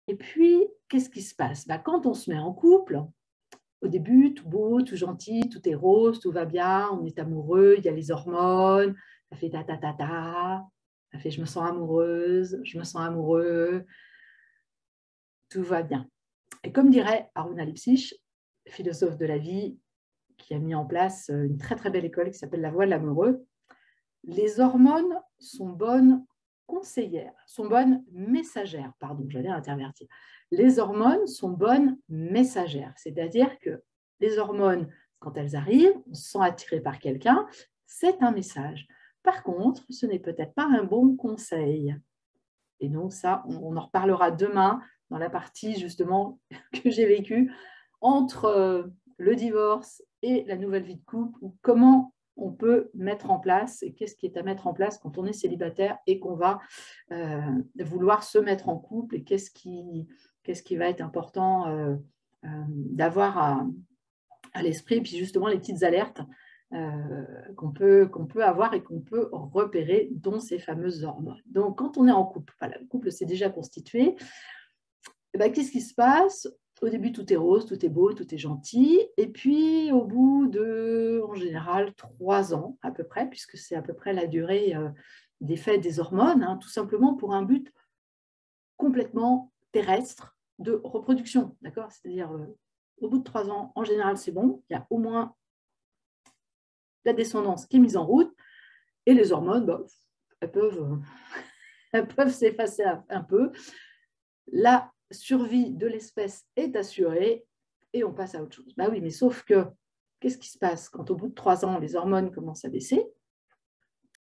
Extrait MasterClass 1ère soirée "Développer le bonheur en couple"
Ne pas s'engueuler en couple, ne pas se prendre la tête et cultiver le bonheur à deux est-ce vraiment possible ?
Vous allez découvrir comment cet exploit est réalisable